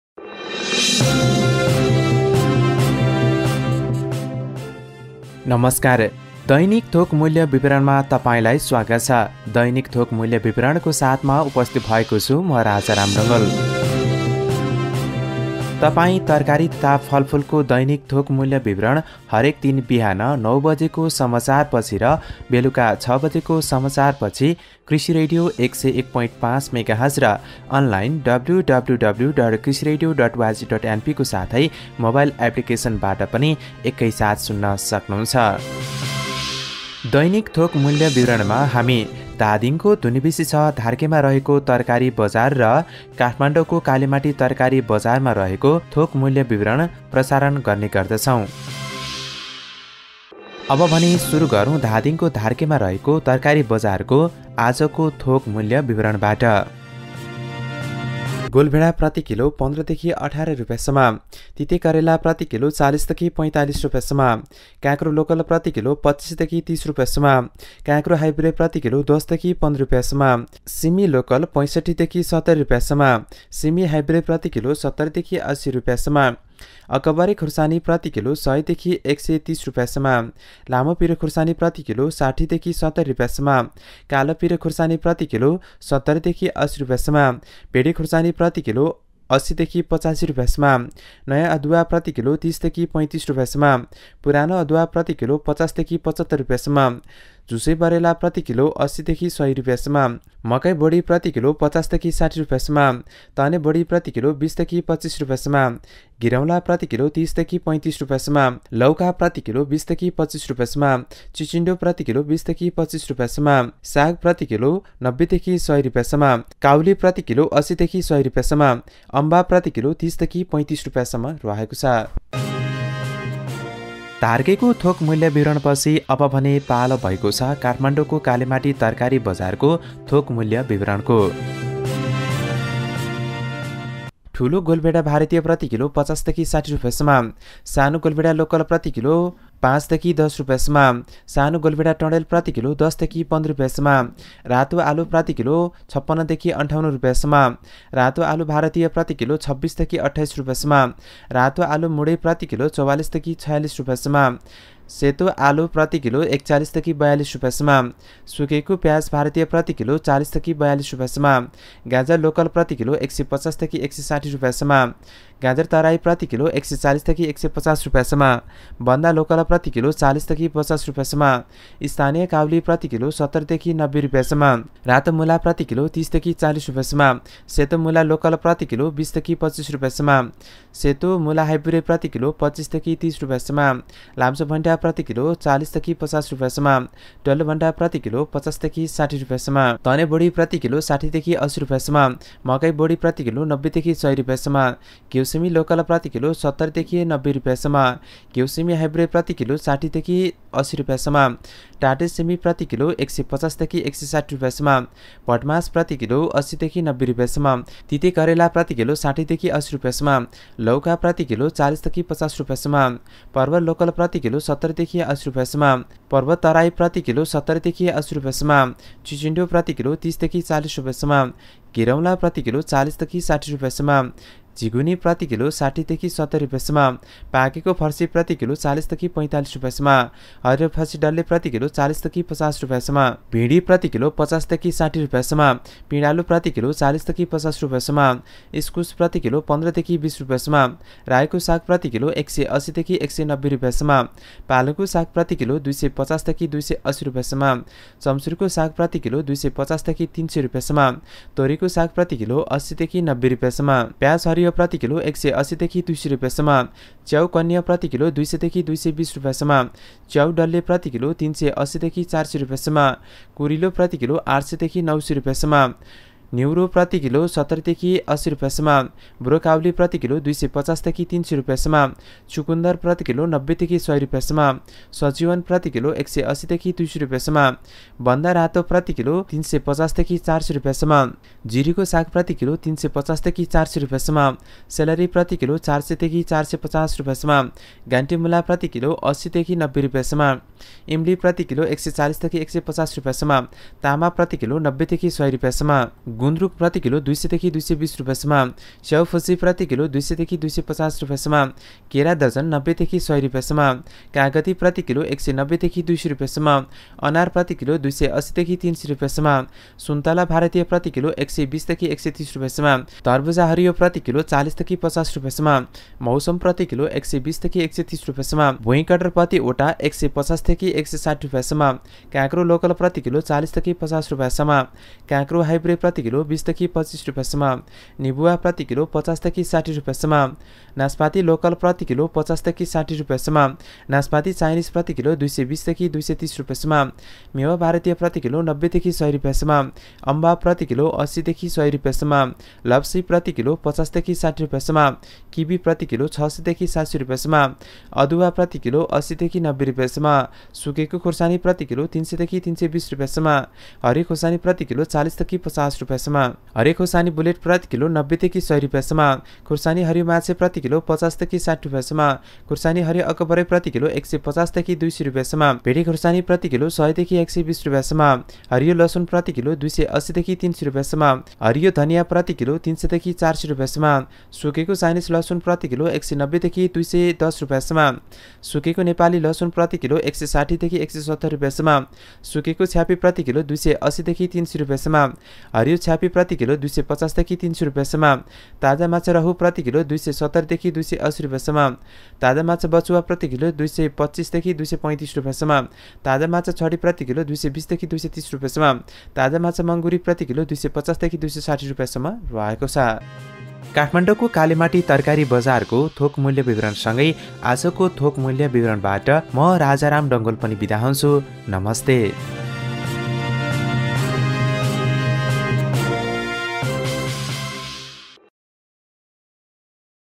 आज भदौ ३०गतेको धार्के र कालिमाटी तरकारी बजारको थोक मूल्य विवरण DhDainik thok mulya bibaran 2078-5-30
आज भदौ ३० गतेको धार्के र कालिमाटी तरकारी बजारको थोक मूल्य विवरण DhDainik thok mulya bibaran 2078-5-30